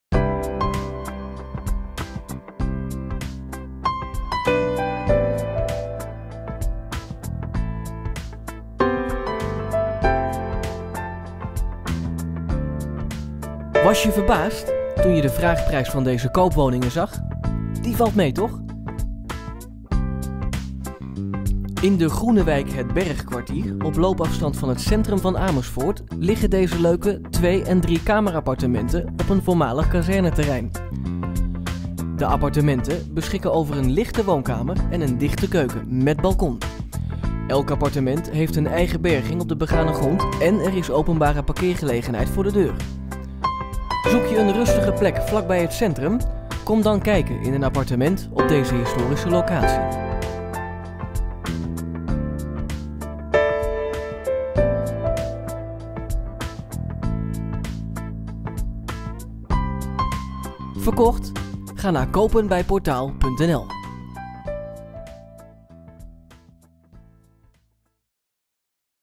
Graaf Willemlaan en Graaf Lodewijklaan
De appartementen bevinden zich op het terrein van de voormalige Juliana van Stolbergkazerne gelegen in de groene wijk “het Bergkwartier”,  op loopafstand van het centrum, park, winkels aan de Leusderweg en nabij openbaar vervoer en uitvalswegen. Parkeren kan pal voor de deur van het appartementencomplex en is openbaar.